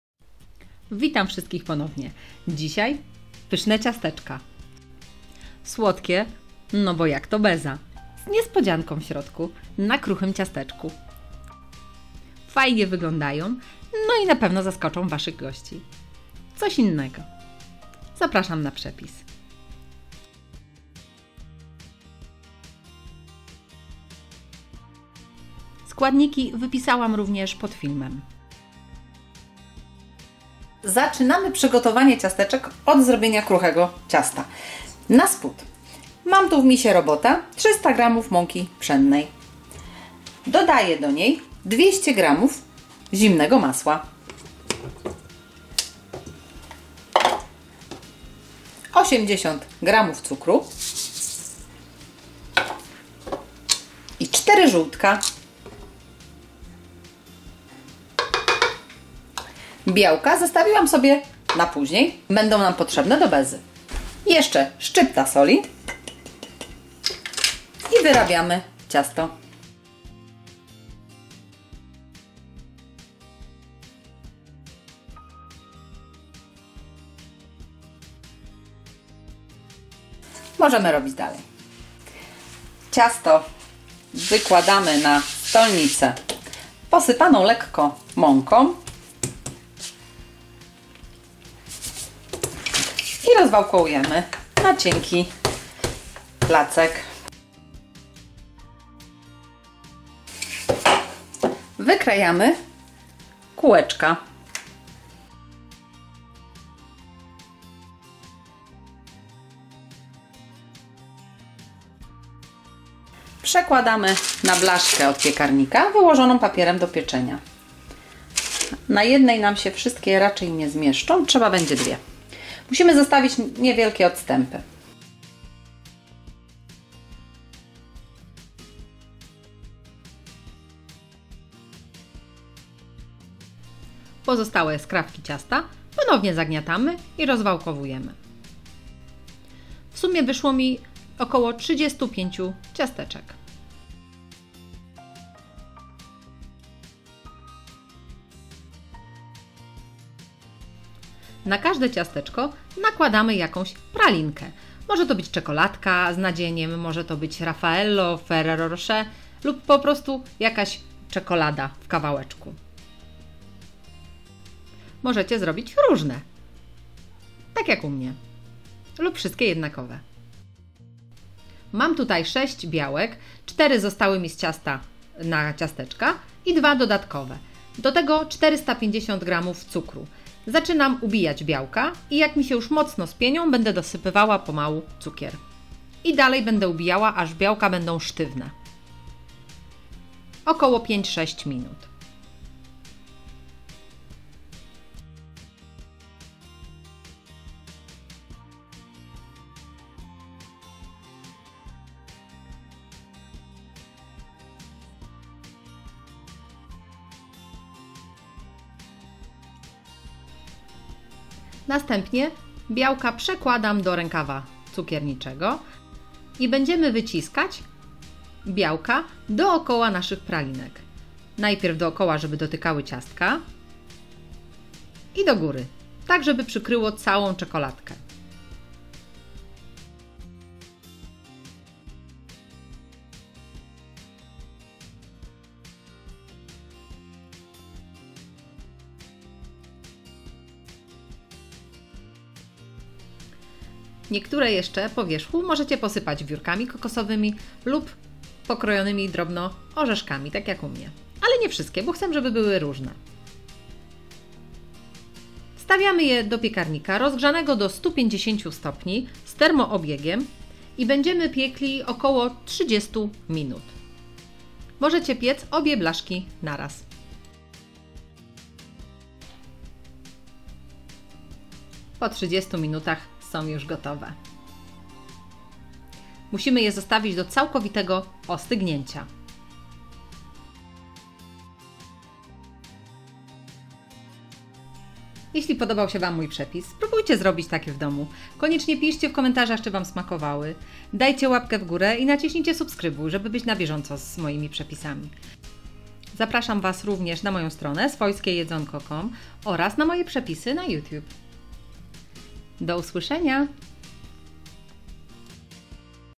Ciasteczka bezowe z niespodzianką - słodkie i pyszne, ciasteczko z bezą i pralinką
Pyszne maślane kruche ciasteczko, z bezą i pralinką schowaną w środku. Na deser, na co dzień i od święta. Wyglądają trochę jak małe choineczki. polecam
Składniki na 35 szt :
4 jajka + 2 białka
450 g cukru (do bezy)
80 g cukru (do ciastek)
200 g masła
300 g mąki
35 pralinek lub czekoladek
Cały przepis na bułeczki w formie tekstowej znajdziecie również na mojej stronie:
Jeśli podobał Ci się mój przepis to proszę
Będzie mi bardzo miło :)
Znajdziecie mnie również na moim fanpage na facebooku :
Zapraszam serdecznie - Gotujmy razem !!